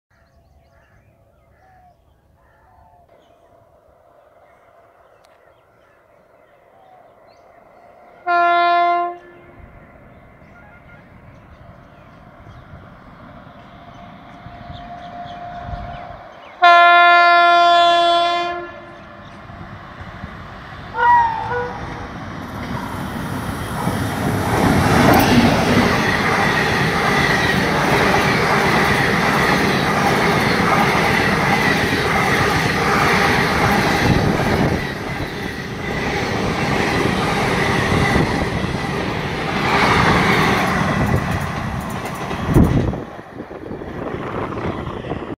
12340 Coalfield Express at mps
12340 Dhanbad Howrah Coalfield Express hauled by WAP 7 at mps
Location -Thaparnagar near Dhanbad